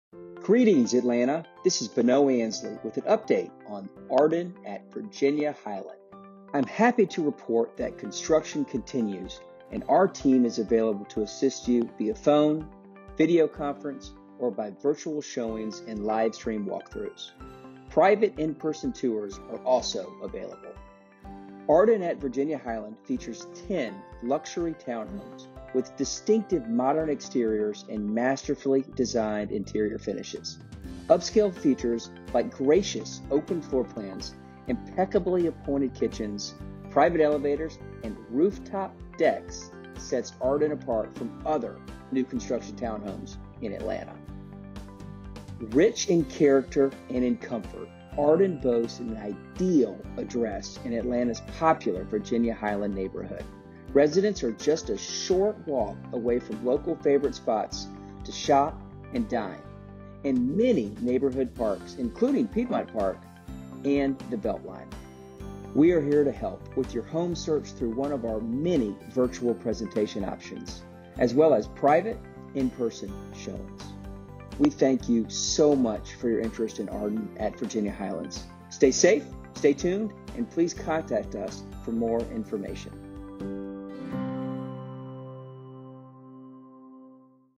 Ardyn VaHi | Spring 2020 Update
Ardyn at Virginia-Highland features 10 luxury townhomes with distinctive modern exteriors and masterfully designed interior finishes. Private, in-person tours or virtual appointments are available. Don't miss out - homes will be completed this Spring.